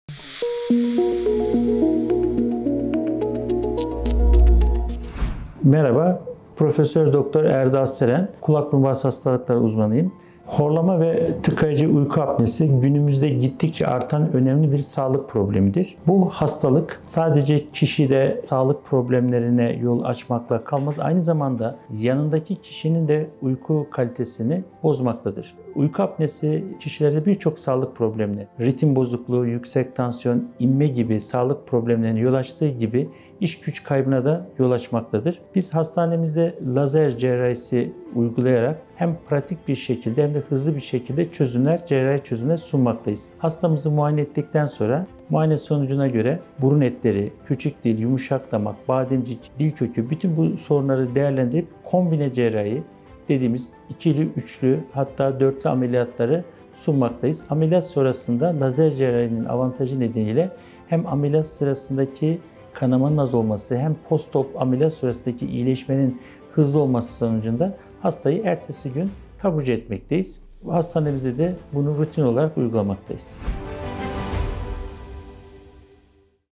Prof. Dr. Erdal Seren - Horlama Cerrahisi
Memorial Ankara Hastanesi Kulak Burun Boğaz Bölümü'nden Prof. Dr. Erdal Seren, horlama cerrahisi konusunda bilinmesi gereken detayları sizler için anlattı.

Horlama Nedir?

Uyku esnasında hava yollarının daralması ve gevşemesi sonucu hava akımının engellenmesi sonrası nefes almanın sert ve gürültülü hale gelmesi durumu horlama olarak tanımlanmaktadır.

Horlama Genel Sağlığı Olumsuz Etkiler

Horlama sadece hastanın değil, onunla aynı odayı paylaşan kişinin de uyku kalitesini ve sağlığını olumsuz etkilemektedir. Kalpte ritim bozukluğu, yüksek tansiyon, inme gibi ciddi hastalıkların yanı sıra günlük hayatta da iş gücü kaybına yol açabilmektedir.

Horlama Cerrahi Olarak Nasıl Tedavi Edilir?

Detaylı bir muayeneden geçirilen hasta burun eti, yumuşak damak, küçük dil, bademcik ve dil kökü açısından değerlendirilerek kombine cerrahiyle (çoklu ameliyat) lazer yöntemi kullanılarak ameliyat edilmektedir. Hasta genel sağlık durumuna göre ertesi gün taburcu olabilmektedir.